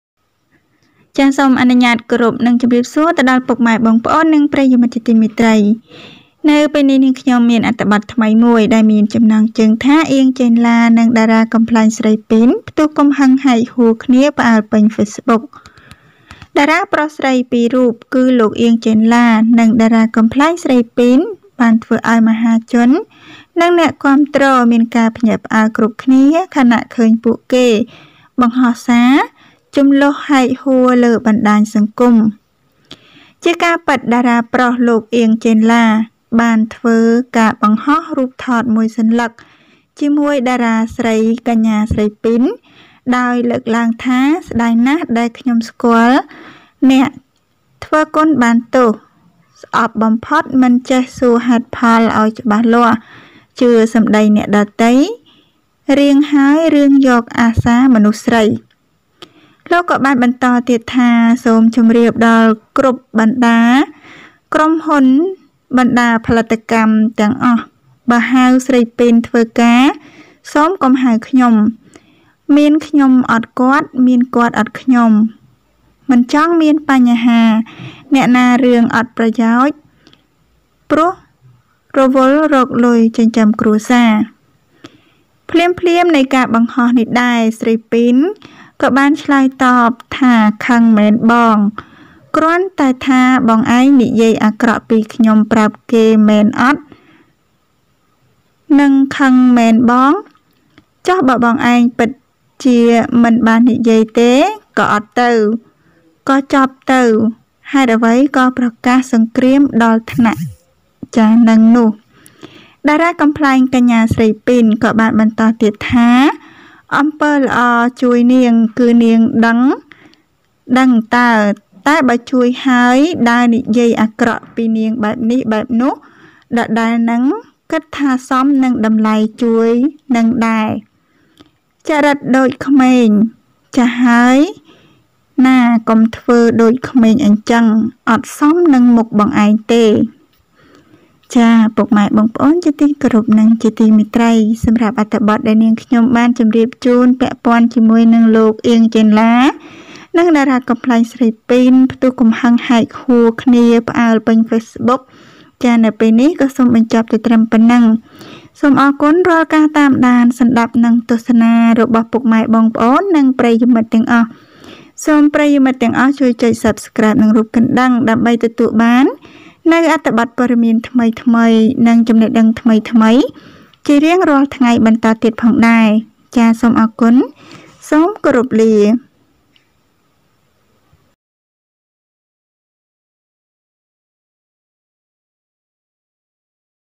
Ieng Chenla dan komedian Pin meledak dalam kemarahan di Facebook
អៀង ចេនឡា និងតារាកំប្លែង ស្រីពីន ផ្ទុះកំហឹងហែកហួរគ្នាផ្អើលពេញហ្វេសប៊ុក